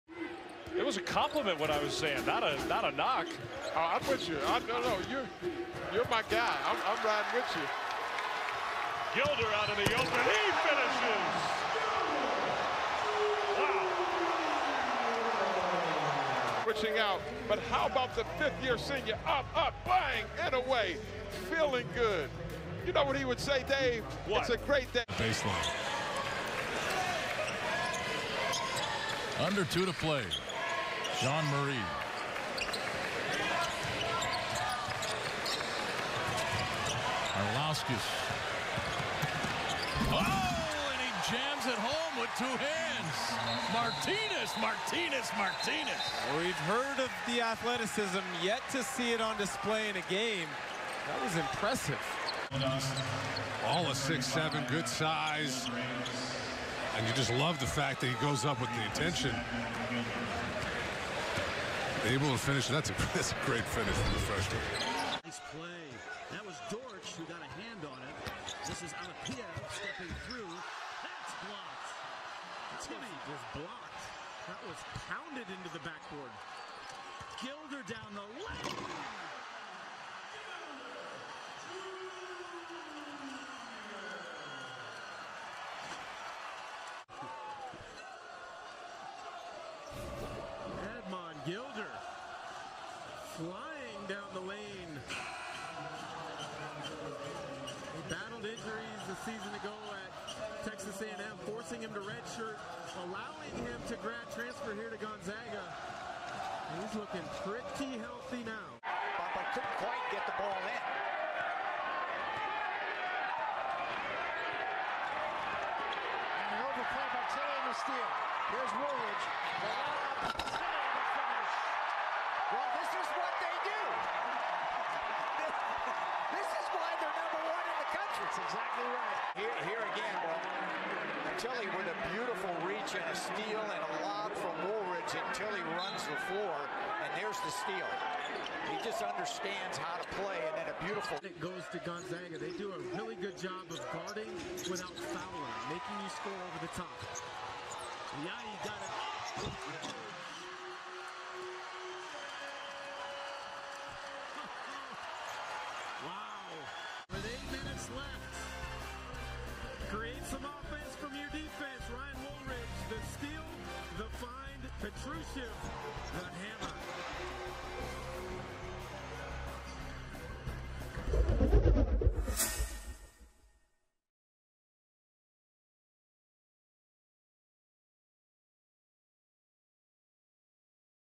Gonzaga Basketball Top 5 Plays of the 2019-20 Season | Stadium
Check out the top five plays from Gonzaga basketball in the 2019-20 season.